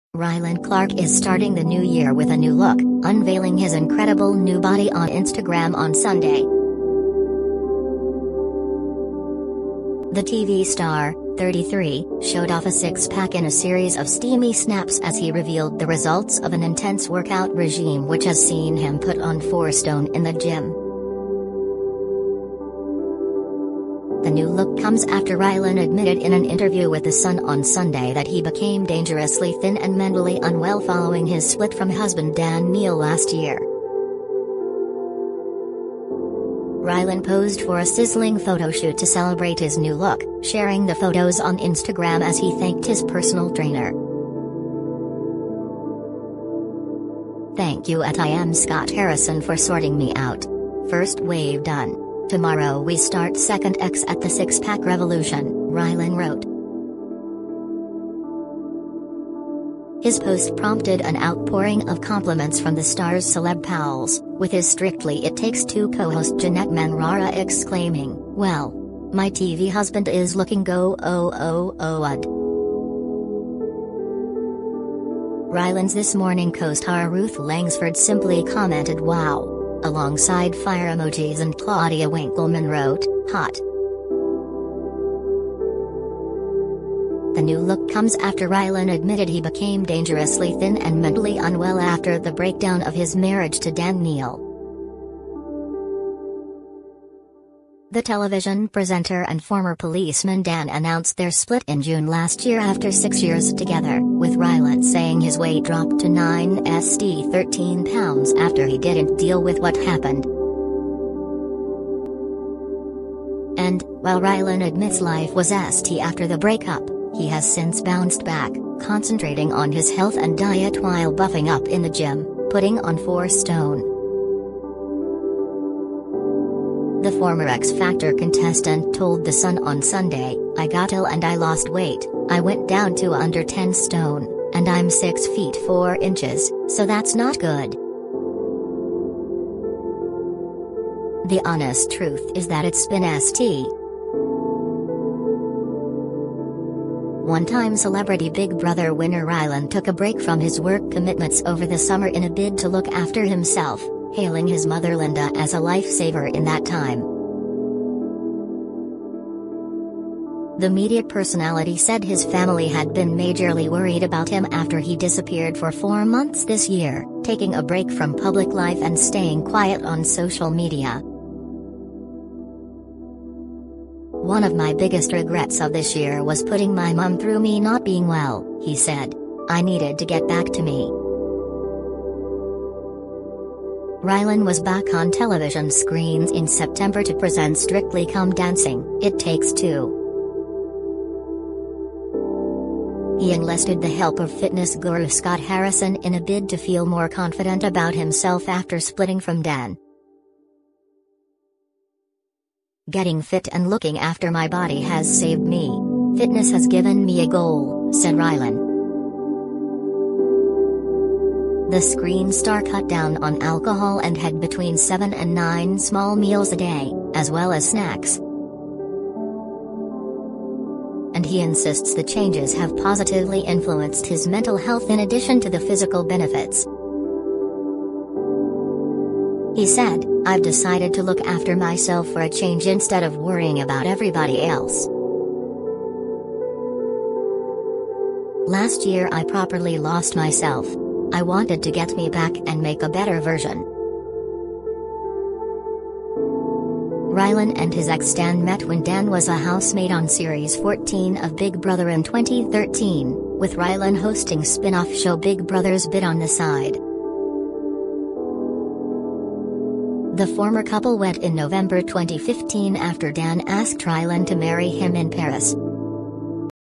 Rylan Clark unveils his incredible six pack and four stone weight gain from new gym regime after adm
Rylan Clark is starting the new year with a new look, unveiling his incredible new body on Instagram on Sunday.
The TV star, 33, showed off a six pack in a series of steamy snaps as he revealed the results of an intense workout regime which has seen him put on four stone in the gym.
The new look comes after Rylan admitted in an interview with The Sun on Sunday that he became 'dangerously thin' and 'mentally unwell' following his split from husband Dan Neal last year.
Rylan posed for a sizz